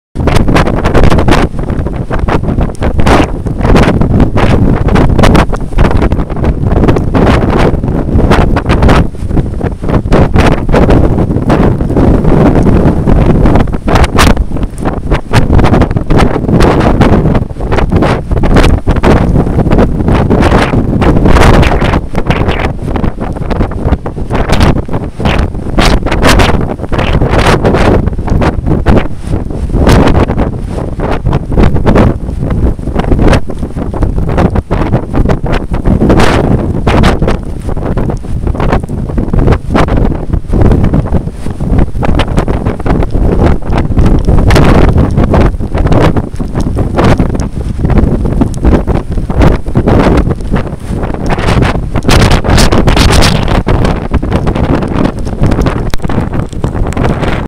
Porthcawl Storm
Just a little video to show the conditions at Porthcawl Beach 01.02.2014 , it was 2 hours before high tide but almost too dark to see anything properly .